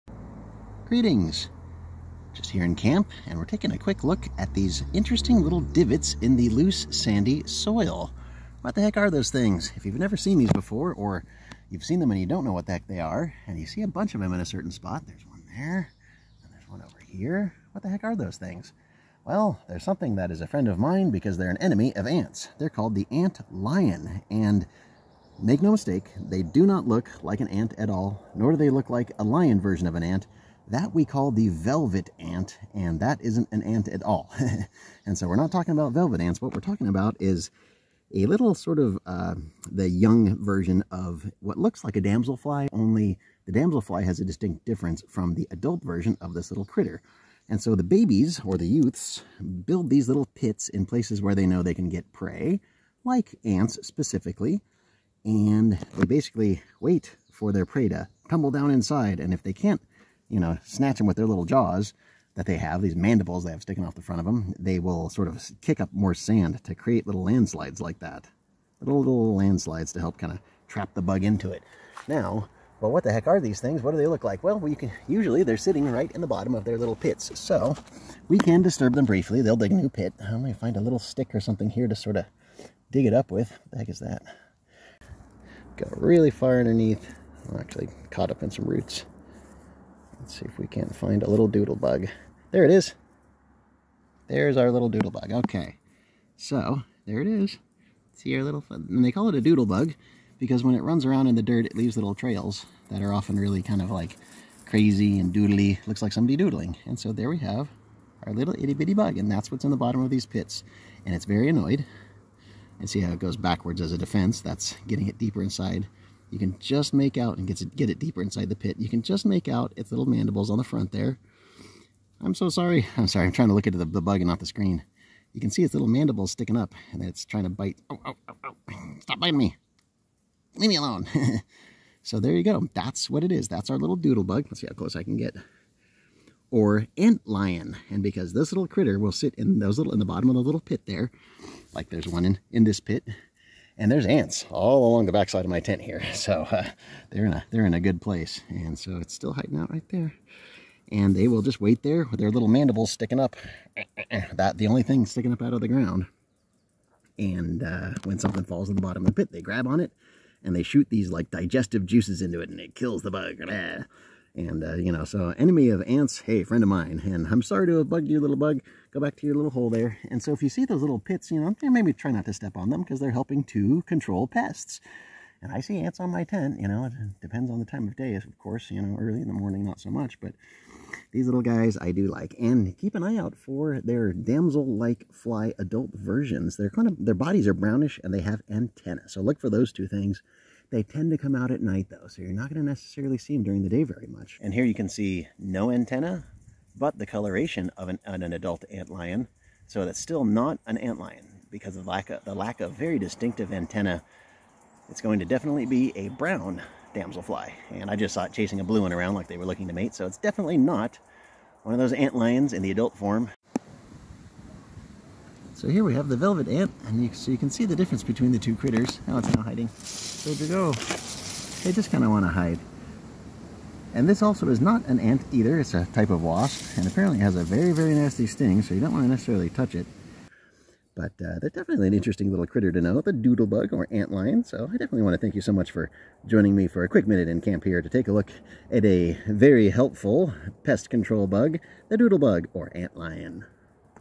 Good Bugs: ant lion/doodlebug - Always Ants
Join me in camp as I take a quick look at a useful and helpful little critter, the ant lion, or doodlebug, in Oregon in July 2025.

Any enemy of ants is a friend of mine! The juvenile version of this insect, of the neuropteran family Myrmeleontidae, digs little pits and then settle in the bottom, only their mandibles exposed as they wait for prey to tumble down in.

The adult version is a damselfly lookalike, but it easily distinguished by its very prominent antennae, which the damselfly does not possess.

If you see these little divots or pits, do try to avoid stepping on them, if only to save the little one the work of having to re-dig its trap. And since they control ants but don't bother us, they're quite welcome anywhere!